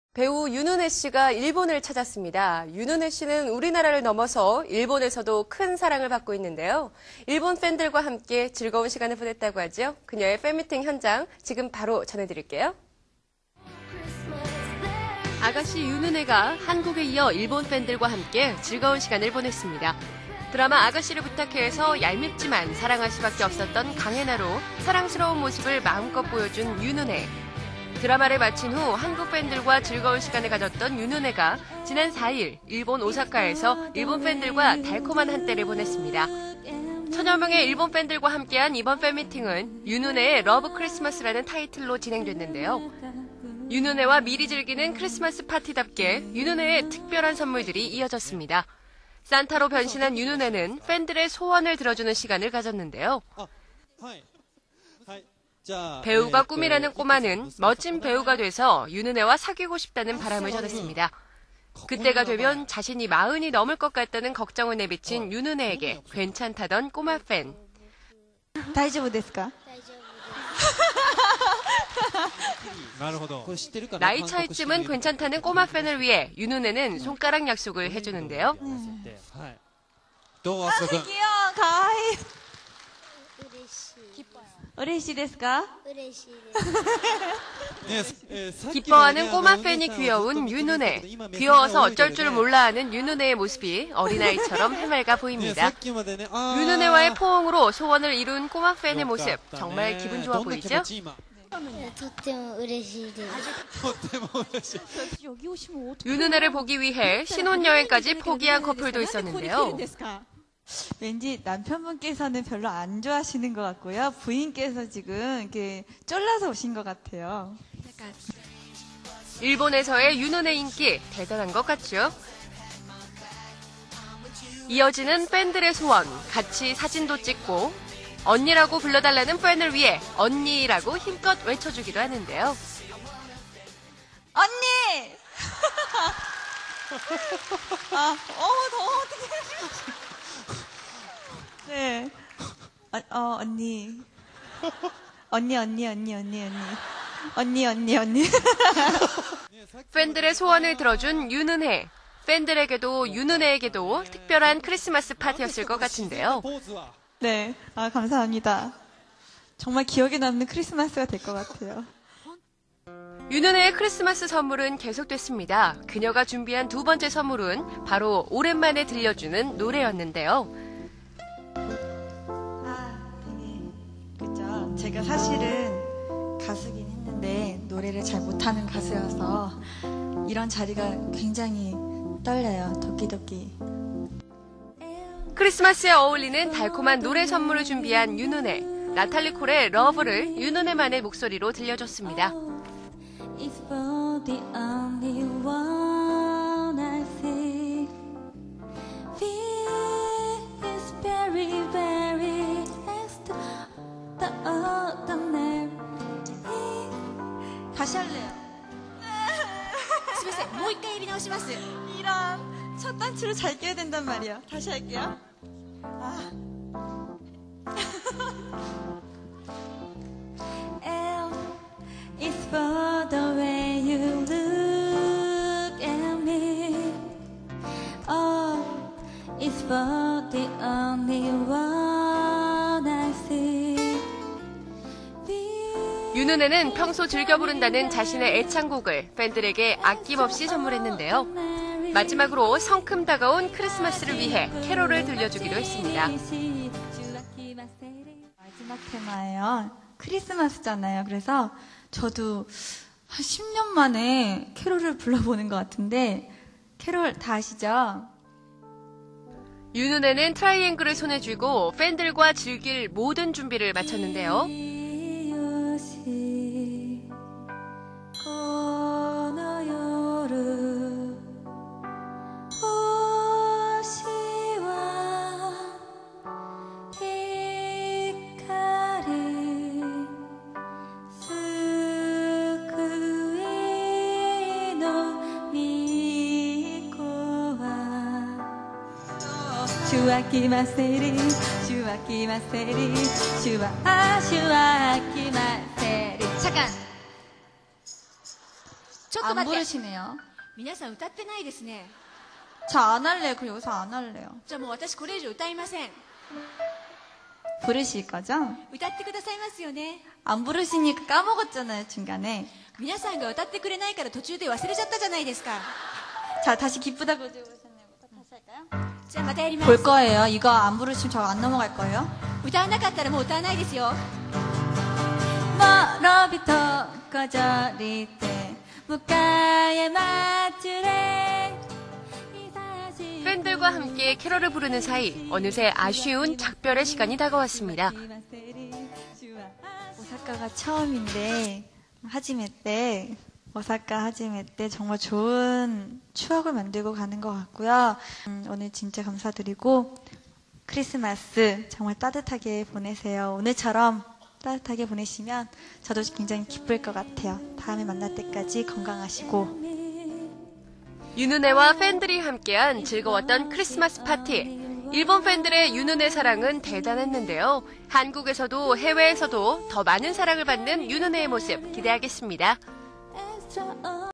Yoon Eun Hye 윤은혜-2009 Japan Fan Meeting
091216

Ystar's coverage on Yoon Eun Hye Japan FM

A young boy proposed to Yoon Eun Hye